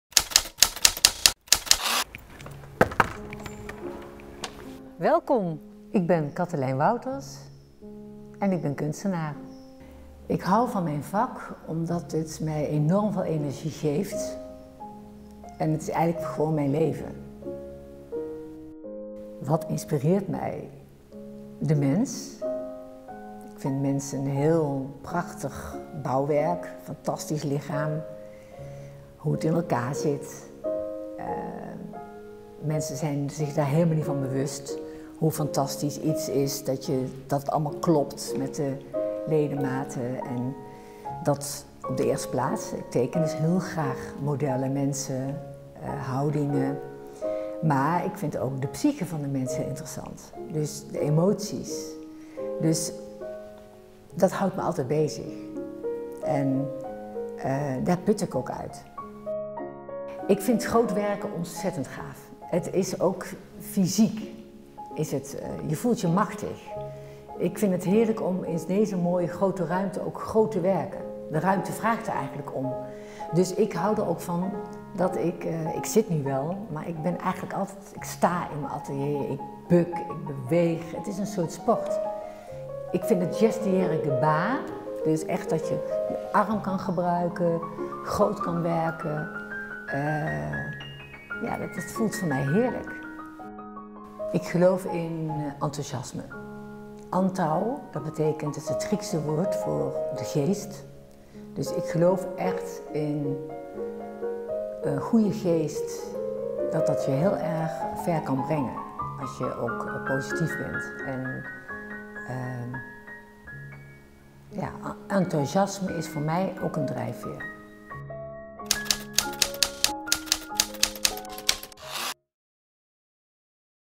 Cathalijn Wouters interview
Cathalijn Wouters, painter (1955), AKV St. Joost Breda, soloshows in New York 2010 and Shanghai 2013, residency in Beijing and New York / Mondriaan Fonds, live drawing at choreographer Hans van Manen at Dutch National Ballet 2013.